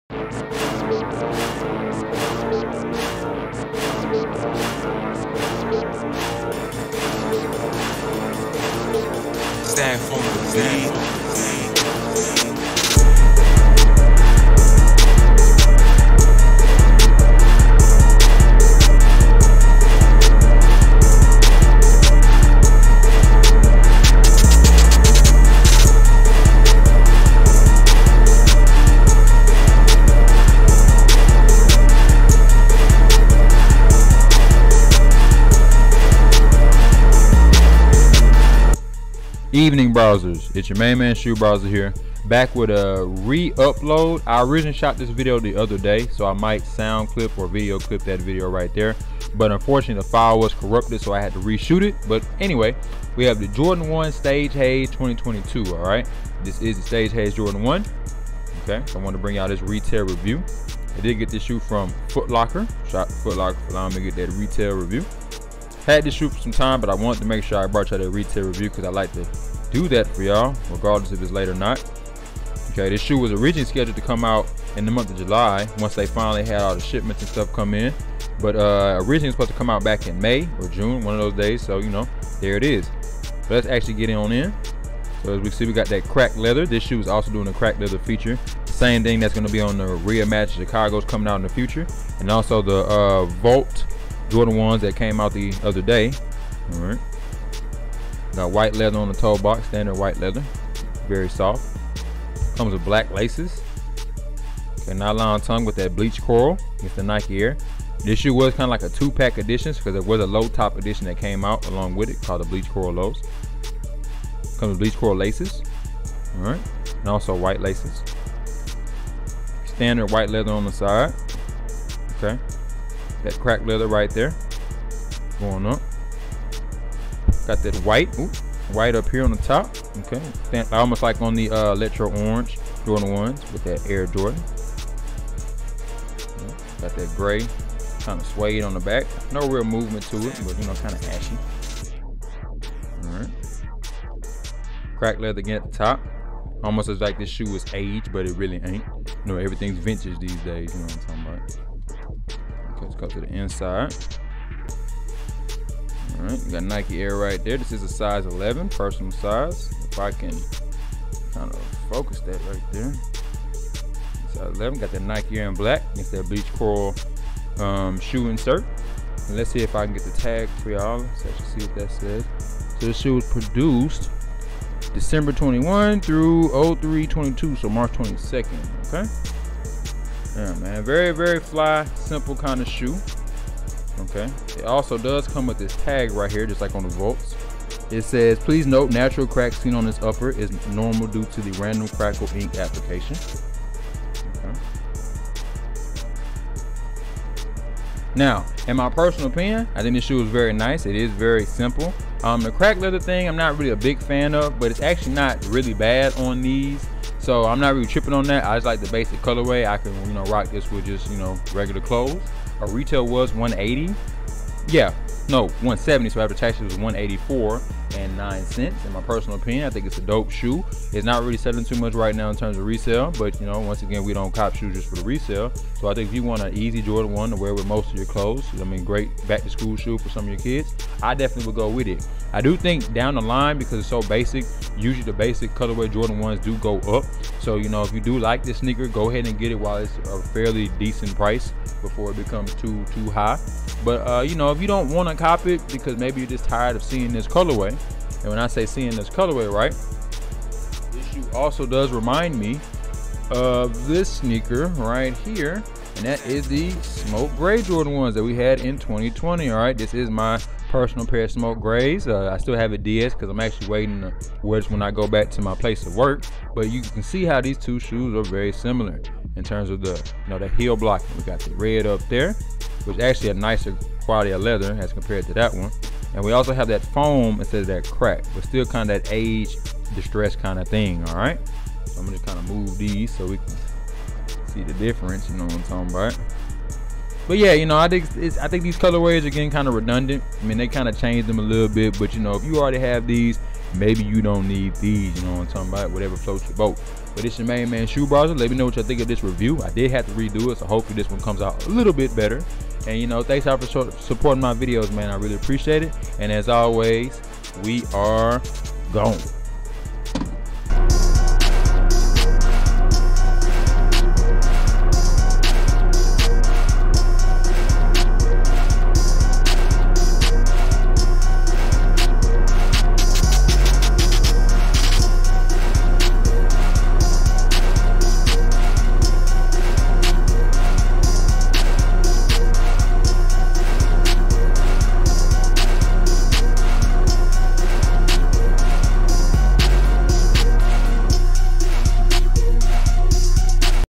[Review 49] - Jordan 1: Stage Haze 2022
Evening Browzers , ya Main Mayne Shoe Browzer is back with another 😴 AJ1. I would’ve had this up for y’all yesterday , but the Browzer team was having technical difficulties.

Either way, the Stage Haze 1’s are clean imo. Did you 👮‍♂️ or 🛌 ?

Stay safe and stay blessed.

Mursiq: @STEPHBEATSRUS 🔥🔥